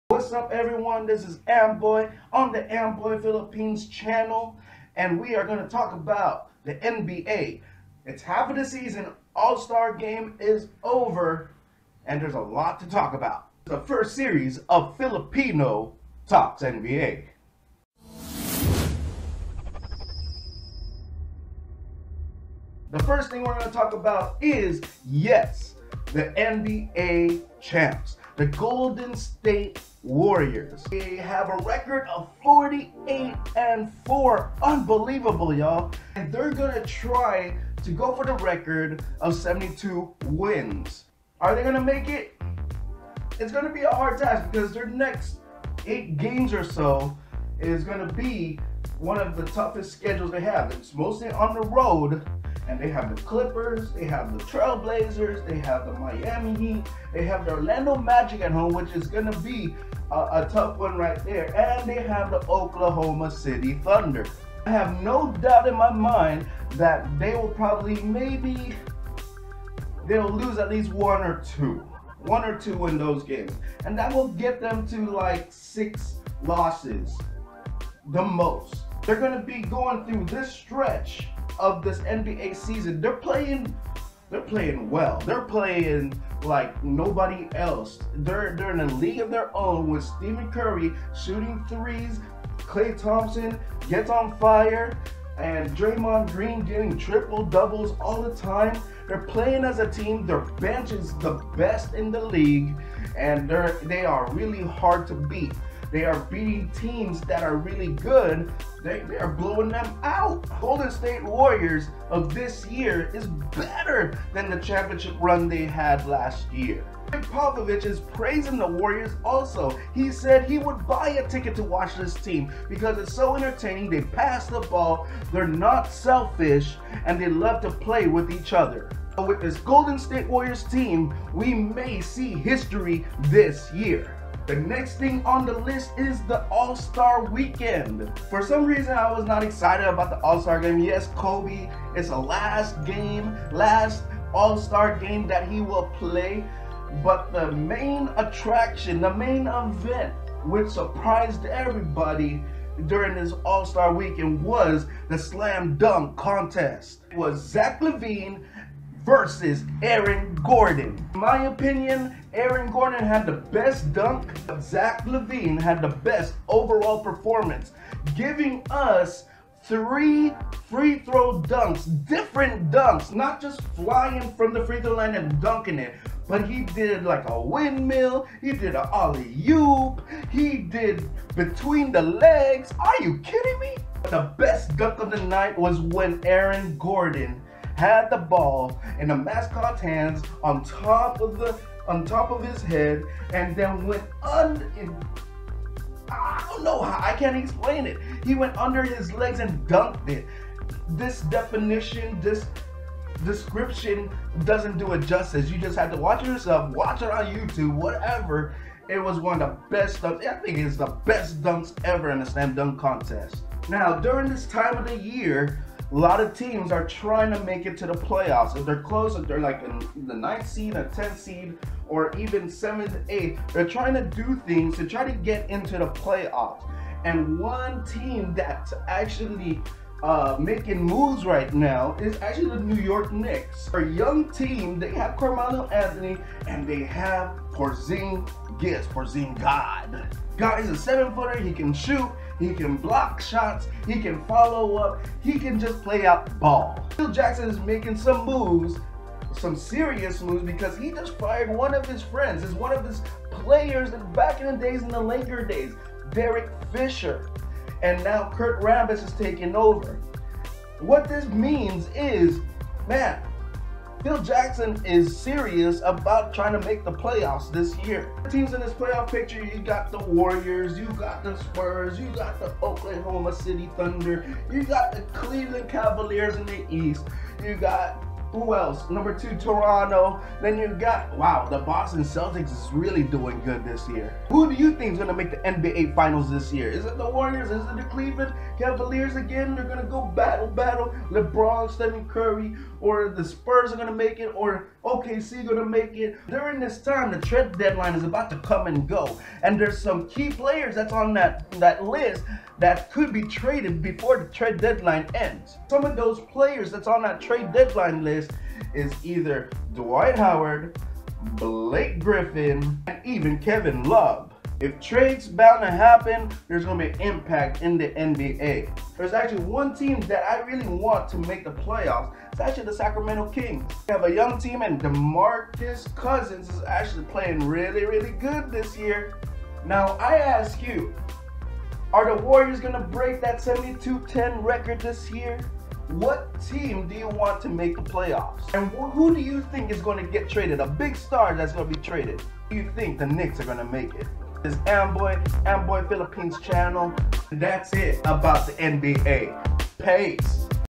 FILIPINO TALKS NBA - Amboy Philippines
Amboy, an American Filipino, talks about the NBA in the halfpoint mark of the 2016 Season.